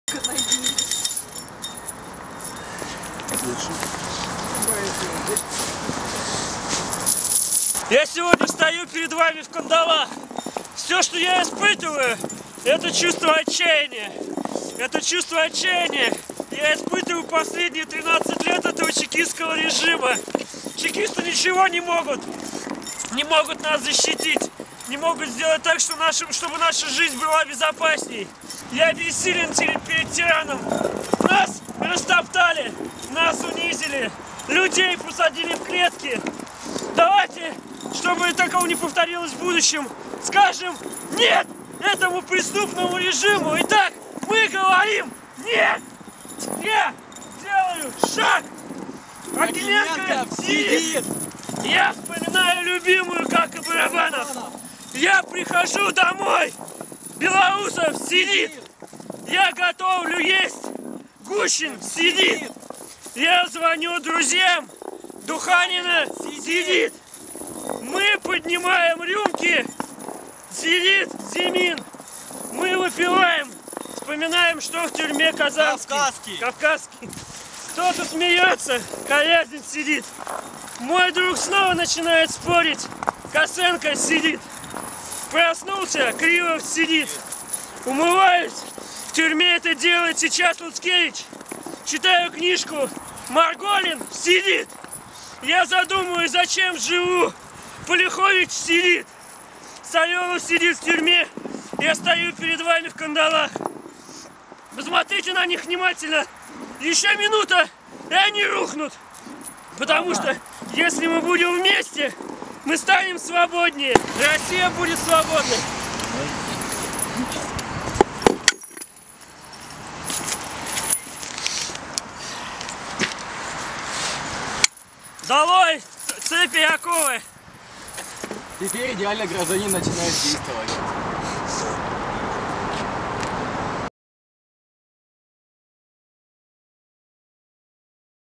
Идеальный гражданин вернулся. Теперь в цепях
Молодежное Яблоко провело акцию "Идеальный гражданин 2" в поддержку политических заключенных